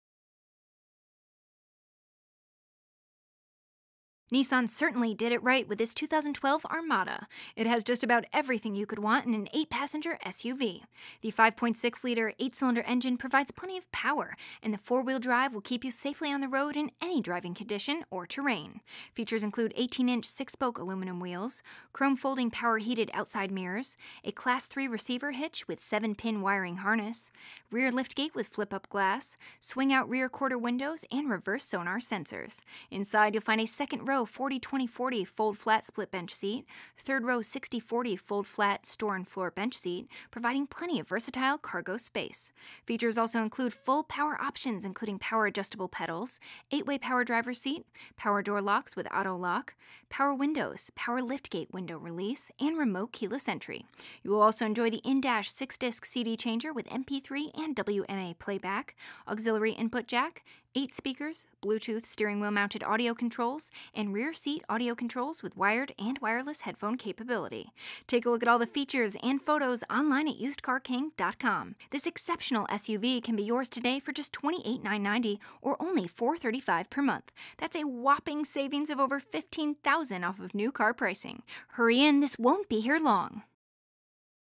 2012 Nissan Armada SV 4WD save and WHOPPING 15K!! Buy from the Used Car King
Take command of the road in the 2012 Nissan Armada!

It just arrived on our lot, and surely won't be here long! With fewer than 35,000 miles on the odometer, this 4 door sport utility vehicle prioritizes comfort, safety and convenience. Nissan prioritized practicality, efficiency, and style by including: a built-in garage door transmitter, adjustable pedals, and cruise control. It features an automatic transmission, 4-wheel drive, and a powerful 8 cylinder engine.

We pride ourselves in consistently exceeding our customer's expectations. Stop by our dealership or give us a call for more information.